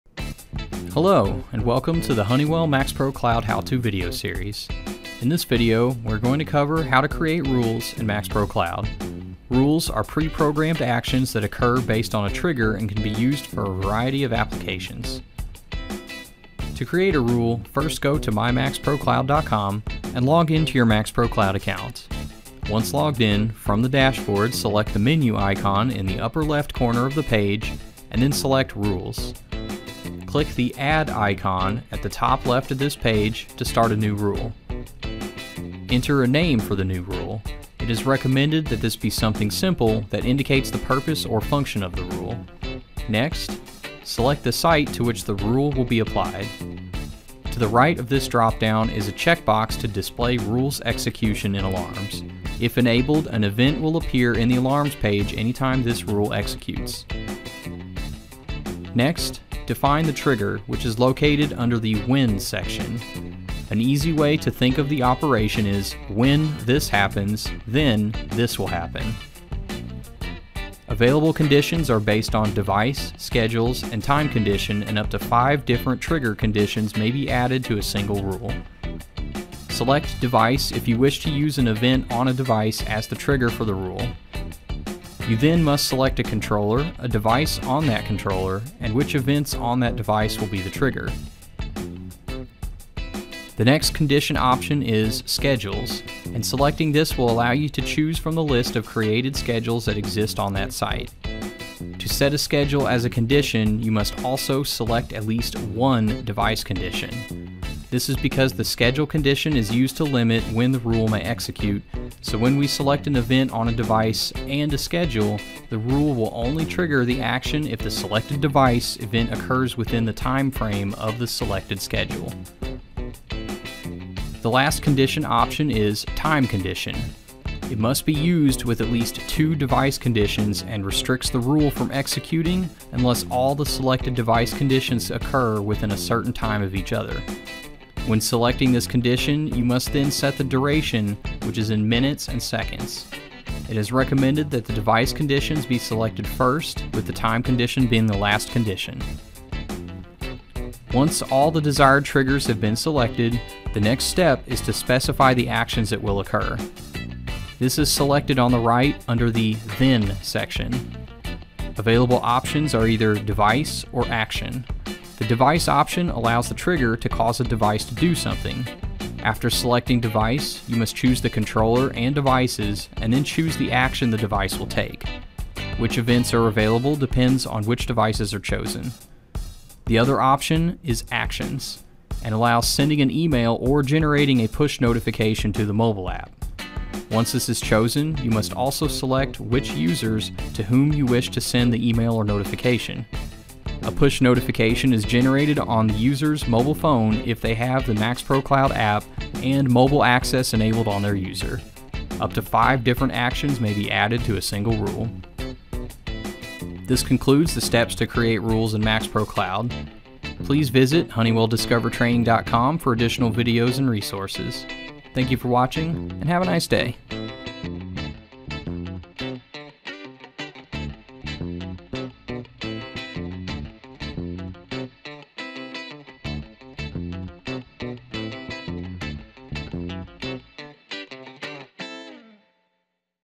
How to Create Rules in MAXPRO Cloud - Honeywell Support
This video explains how to create rules in MAXPRO® Cloud.